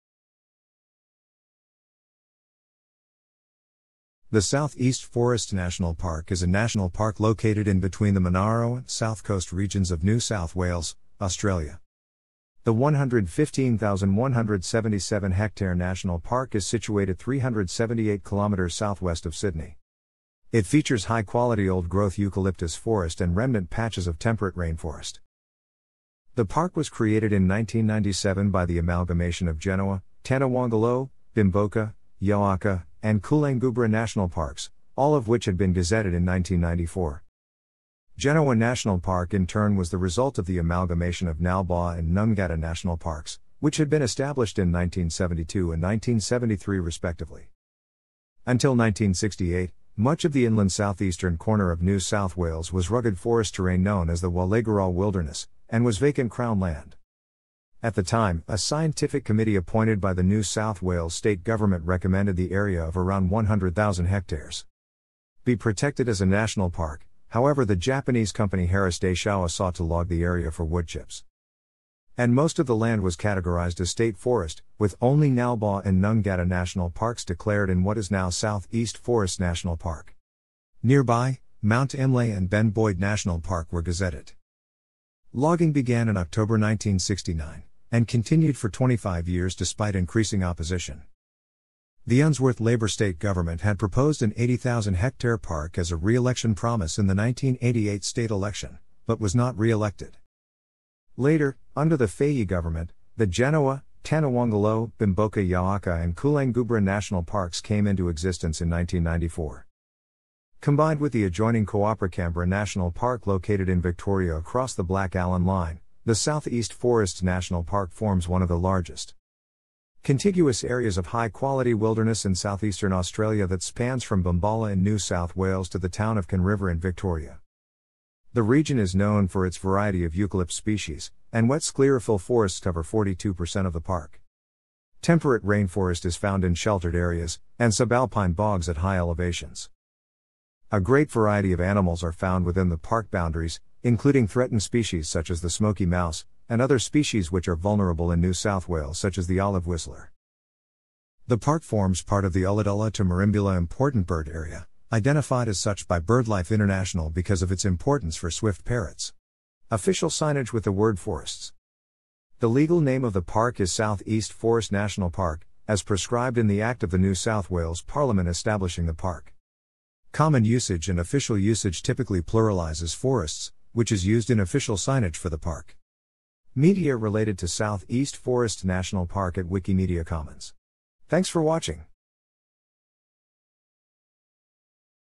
South East Forests National Park
South_Coast_(New_South_Wales)
The South East Forests National Park is a national park located in between the Monaro and South Coast regions of New South Wales, Australia.
The 115,177-hectare （284,610-acre） national park is situated 378 kilometres （235 mi） southwest of Sydney.
It features high quality old growth eucalyptus forest and remnant patches of temperate rainforest.
The park was created in 1997 by the amalgamation of Genoa, Tantawangalo, Bemboka, Yowaka and Coolangubra National Parks, all of which had been gazetted in 1994.
Genoa National Park in turn was the result of the amalgamation of Nalbaugh and Nungatta National Parks, which had been established in 1972 and 1973 respectively.
Until 1968, much of the inland southeastern corner of New South Wales was rugged forest terrain known as the Wallagaraugh Wilderness, and was vacant crown land.
At the time, a scientific committee appointed by the New South Wales state government recommended the area of around 100,000 hectares
（250,000 acres） be protected as a national park, however the Japanese company Harris-Daishowa sought to log the area for woodchips,
and most of the land was categorised as state forest, with only Nalbaugh and Nungatta National Parks declared in what is now South East Forests National Park.
Nearby, Mount Imlay and Ben Boyd National Park were gazetted.
Logging began in October 1969, and continued for 25 years despite increasing opposition.
The Unsworth Labor State government had proposed an 80,000-hectare （200,000-acre） park as a reelection promise in the 1988 state election, but was not reelected.
Later, under the Fahey government, the Genoa, Tantawangalo, Bemboka Yowaka and Coolangubra national parks came into existence in 1...